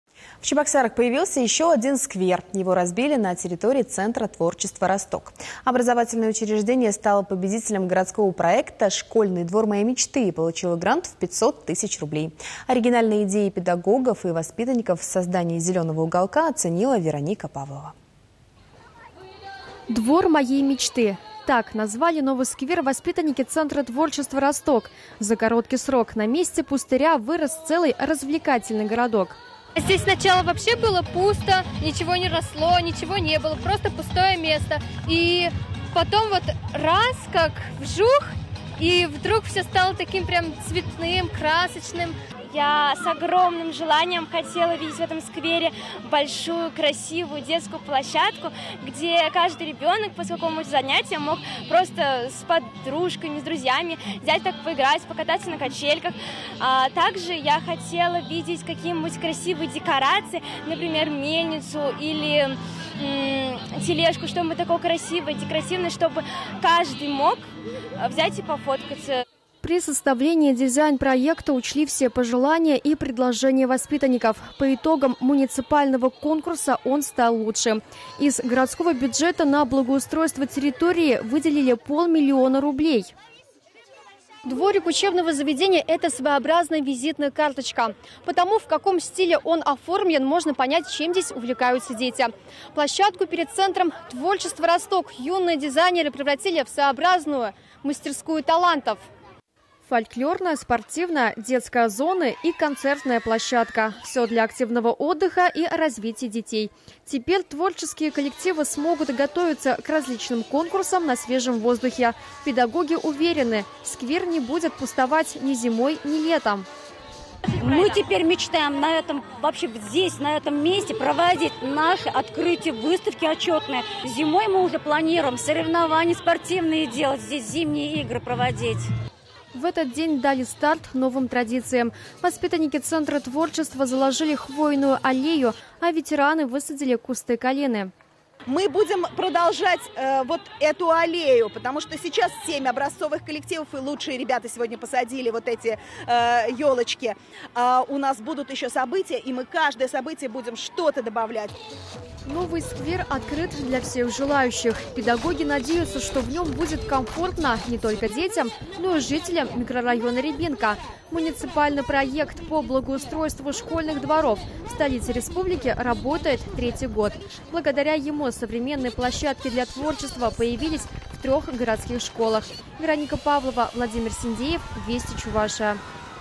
Ещё один сквер появился в Чебоксарах
В Чебоксарах появился еще один сквер. Его разбили на территории центра творчества «Росток».